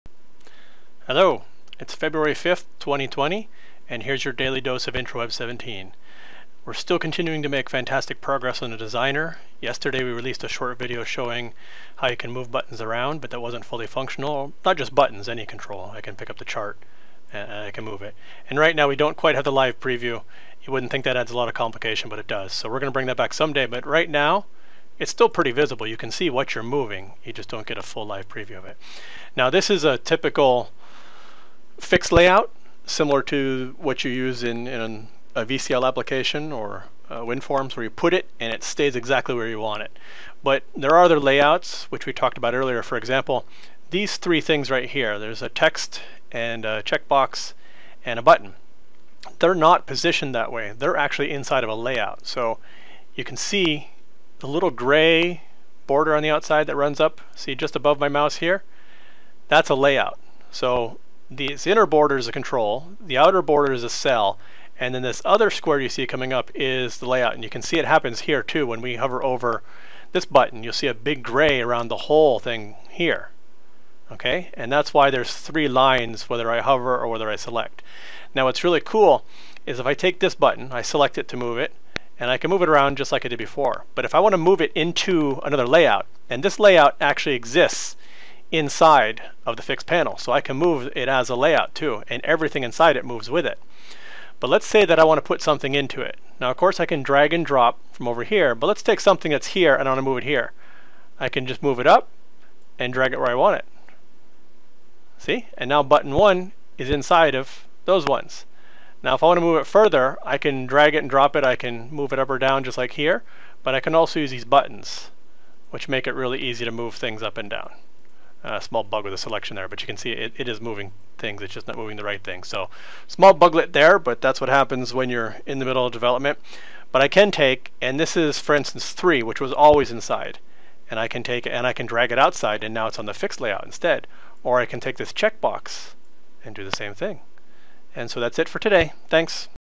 IW17 Designer Feb 5, 2020
See drag and drop between varying layouts and other new features.